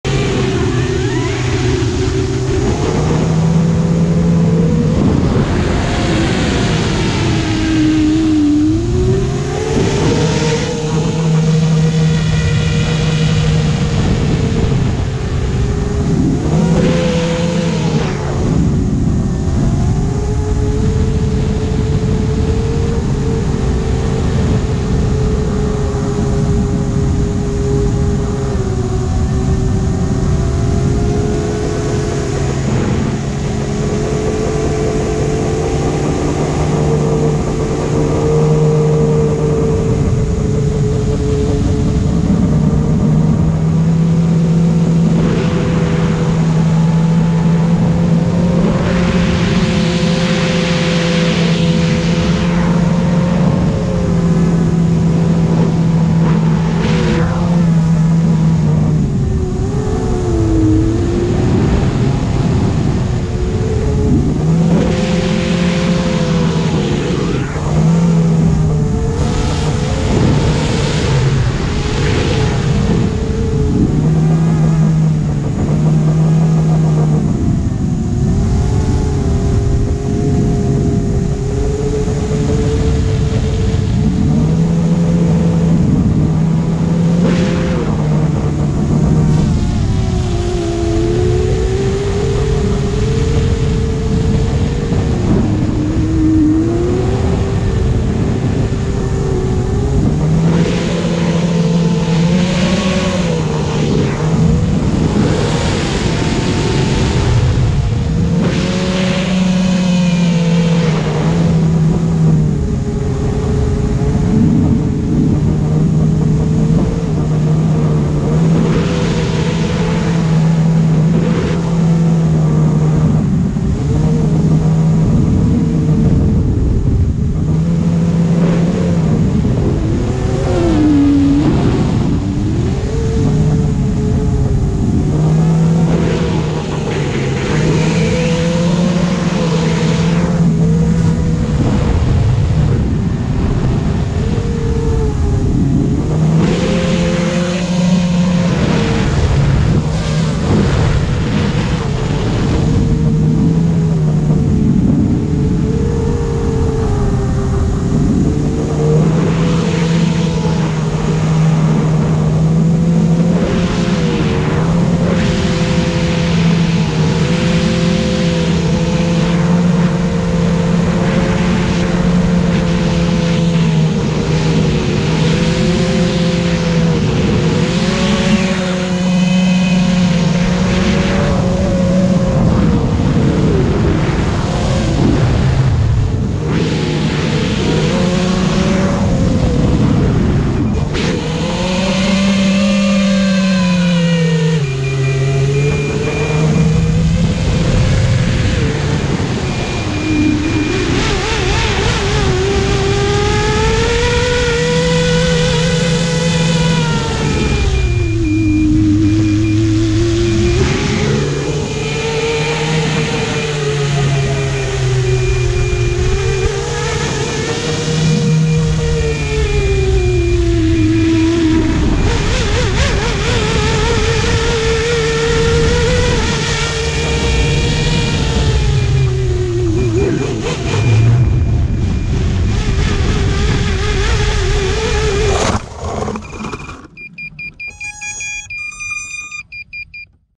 20220217Betaflight Rescue Mode - Drone rakitan jadi bisa pulang sendiri
Rescue mode aktif di menit 3:08

Rescue mode kali ini aktif ketika module receiver / RX eror tiba-tiba sinyal ilang.
Alhamdulilah otomatis balik walo titik landingnya geser 10 meteran :D